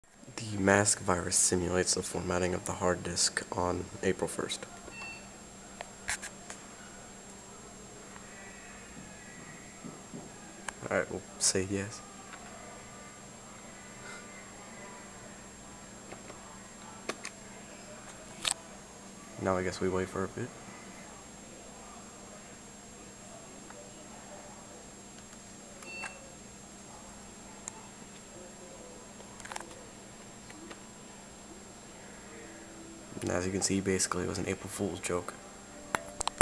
Mask DOS Virus
The Mask virus activates on April 1st and simulates a formatting of the hard disk. Now isn't that a much better April Fools virus than damn Conficker?